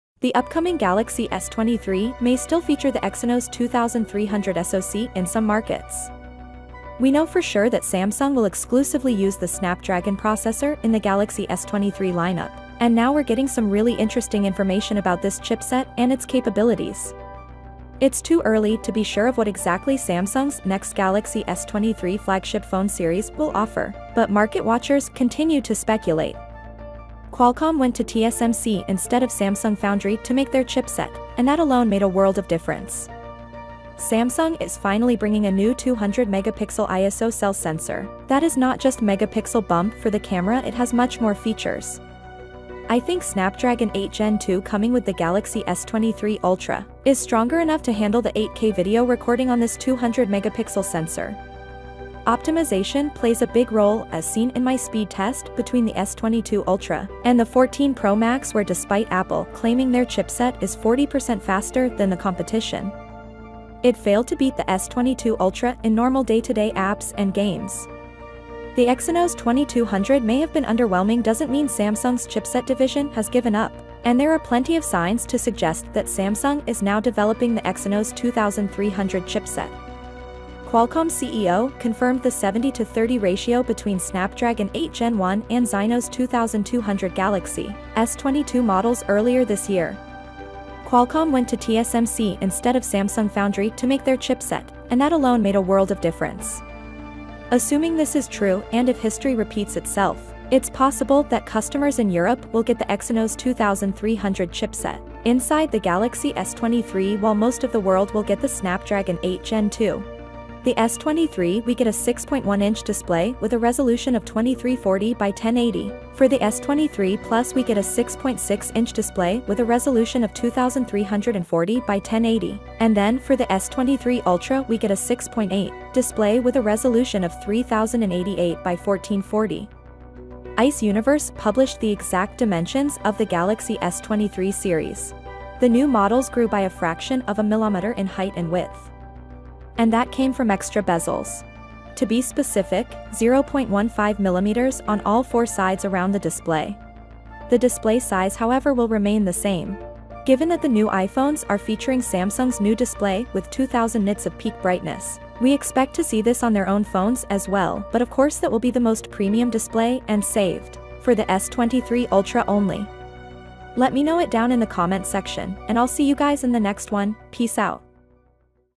Samsung Galaxy S23 Ultra - SURPRISE FROM SAMSUNG
The upcoming galaxy s23 may still feature the exynos 2300 soc in some markets. We know for sure that Samsung will  exclusively use the Snapdragon processor in the Galaxy S23 lineup, and now we're getting  some really interesting information about this chipset and its capabilities. It's too early to be sure of what exactly samsung's next galaxy s23 flagship phone series will offer, but market watchers continue to speculate. Qualcomm went to TSMC  instead of Samsung foundry to make their chipset, and that alone made a world of difference. Samsung is finally bringing a new 200 megapixel iso cell sensor, that is not just megapixel bump for the camera it has much more features. I think snapdragon 8gen2 coming with the galaxy s23 ultra is stronger enough to handle the 8k video recording on this 200 megapixel sensor. Optimization plays a big role as seen in my speed test between the S22 Ultra, and the 14 Pro Max where despite Apple claiming their  chipset is 40% faster than the competition. It failed to beat the S22 Ultra in  normal day-to-day apps and games. The exynos 2200 may have been underwhelming doesn't mean samsung's chipset division has given up, and there are plenty of signs to suggest that samsung is now developing the exynos 2300 chipset. Qualcomm's ceo confirmed the 70 to 30 ratio between snapdragon 8gen 1 and xyno's 2200 galaxy s22 models earlier this year. Qualcomm went to TSMC  instead of Samsung foundry to make their chipset, and that alone made a world of difference. Assuming this is true and if history repeats itself, it's possible that customers in europe will get the exynos 2300 chipset inside the galaxy s23 while most of the world will get the snapdragon 8 gen 2. The s23 we get a 6.1 inch display with  a resolution of 2340x1080 for the s23 plus we get a 6.6" display with a resolution of 2340 by 1080, and then for the s23 ultra we get a 6.8" display with a resolution of 3088 by 1440. Ice Universe published the exact dimensions of the Galaxy  S23 series. The new models grew by a fraction of a millimeter in height and width. And  that came from extra bezels. To be specific, 0.15mm on all four sides around the display. The  display size however will remain the same. Given that the new iphones are featuring  samsung's new display with 2,000 nits of peak brightness, we expect to see this on their own  phones as well but of course that will be the most premium display and saved for the S23 ultra only. let me know it down in the comment section, and i'll see you guys in the next one, peace out.